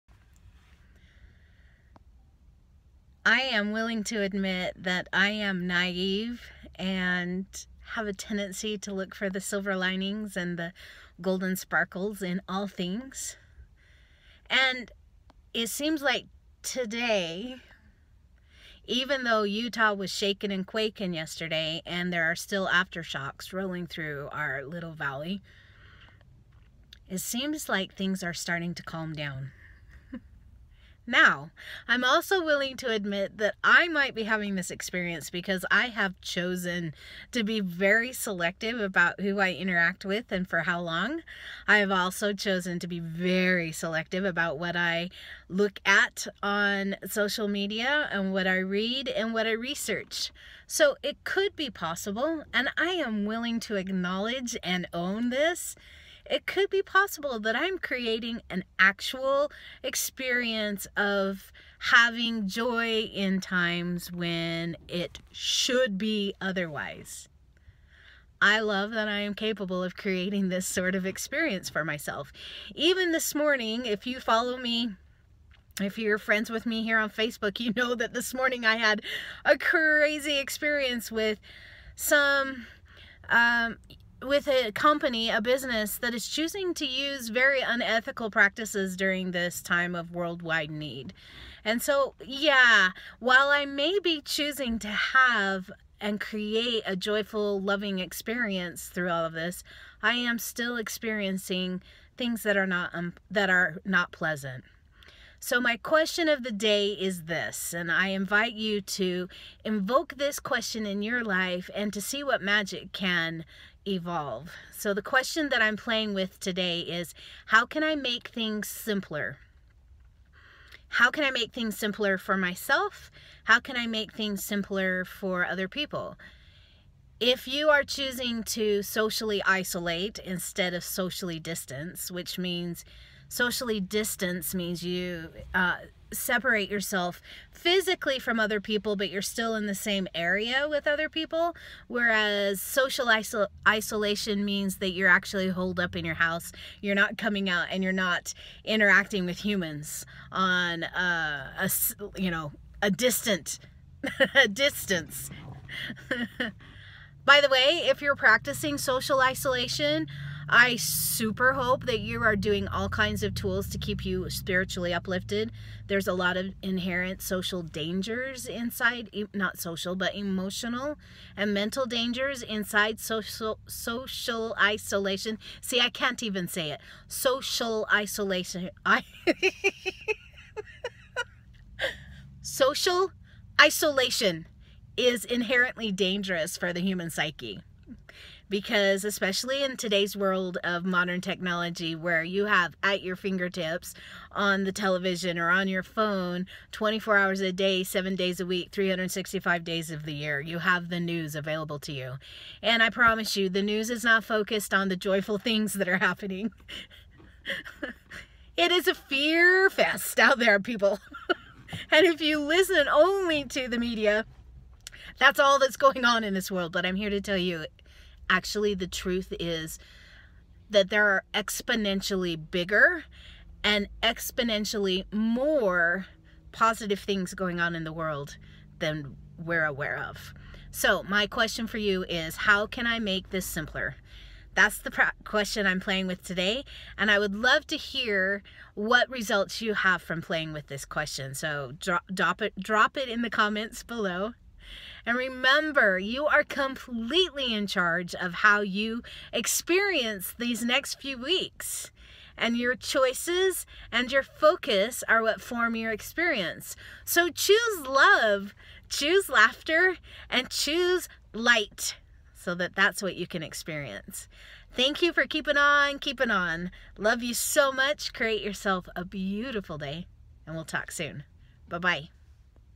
What is the Question of The Day for Thriving in Uncertain Times?
The recent earthquake in Utah combined with the quarantine for the virus has many local EPIC Sensitives questioning "What the heck?!" I've hit the point where laughing is necessary and have begun coming up with new questions to use for moving through the density that is gathering. I'll share both of these with you in today's video.

🔥If you’re interested in learning more about my work, visit my